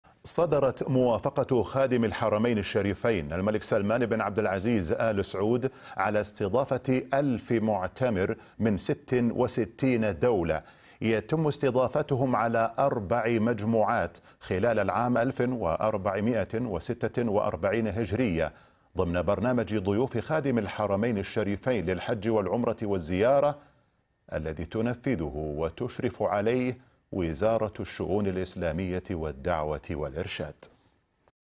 صدور موافقة خادم الحرمين الشريفين على استضافة ألف معتمر من 66 دولة
صدرت موافقة خادم الحرمين الشريفين الملك سلمان بن عبدالعزيز آل سعود
على استضافة 1000 معتمر من 66 دولة، يتم استضافتهم على 4 مجموعات خلال العام 1446هـ، ضمن برنامج ضيوف خادم الحرمين الشريفين للحج والعمرة والزيارة الذي تنفذه وتشرف عليه وزارة الشؤون الإسلامية والدعوة والإرشاد.